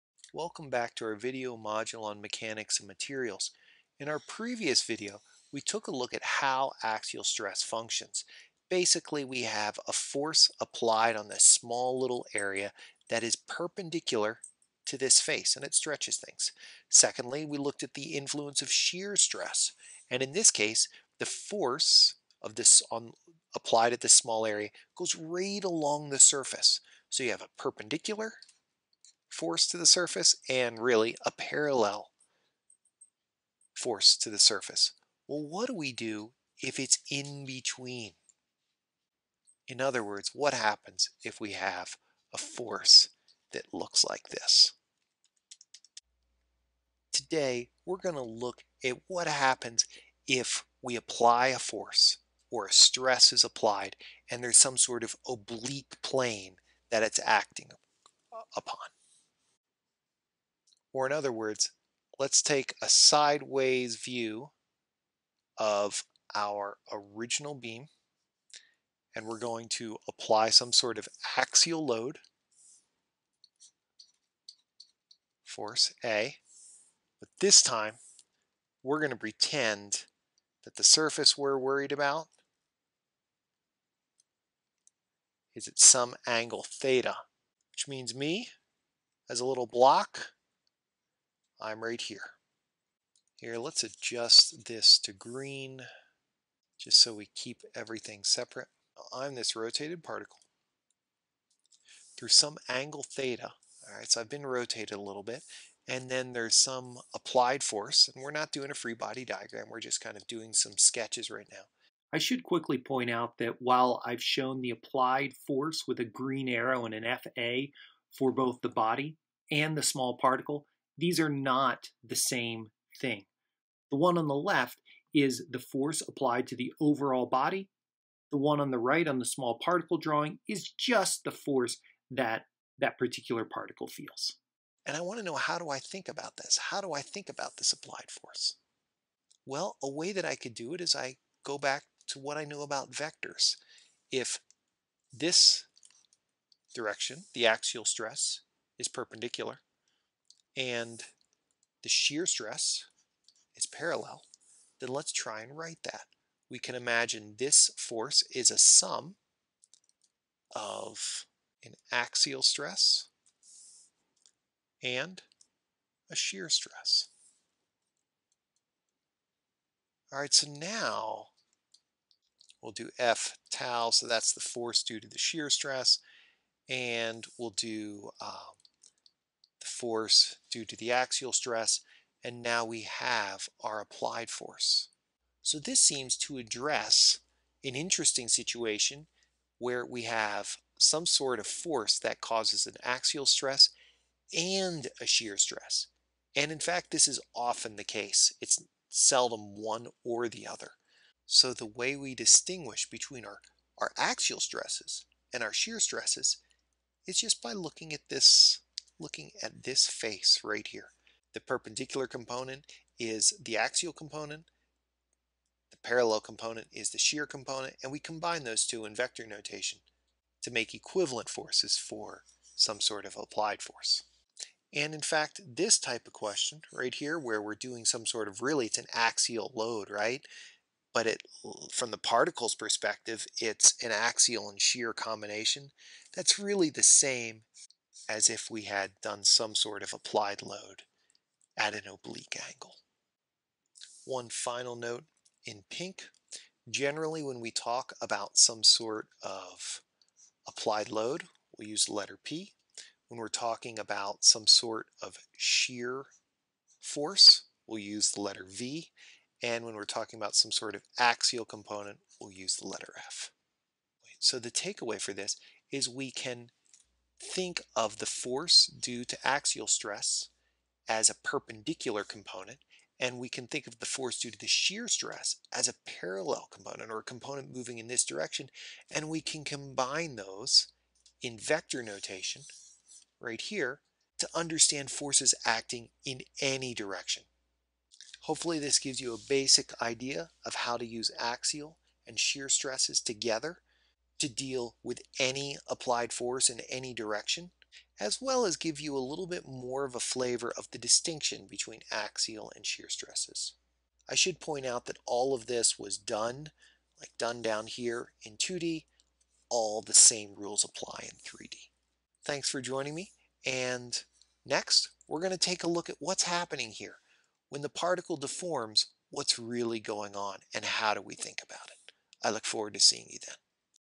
MM 1-04 Oblique Stress
I introduce oblique stresses-- combinations of axial and shear stress.